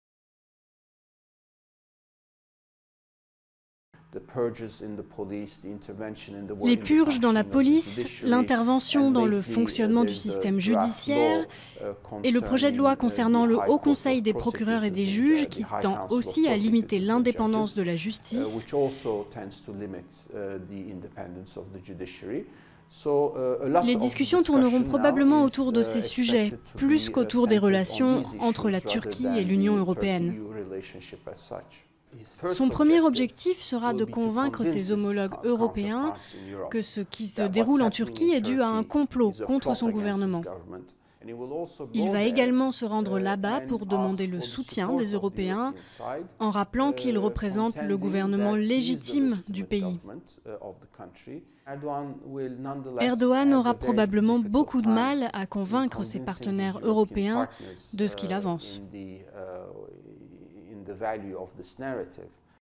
Turquie: Erdogan attendu à Bruxelles
Le Premier ministre turc Recep Tayyip Erdogan, empêtré dans la plus grave crise politique de son règne de onze ans sur la Turquie, se rend lundi 20 janvier en visite à Bruxelles. Durée: 01:03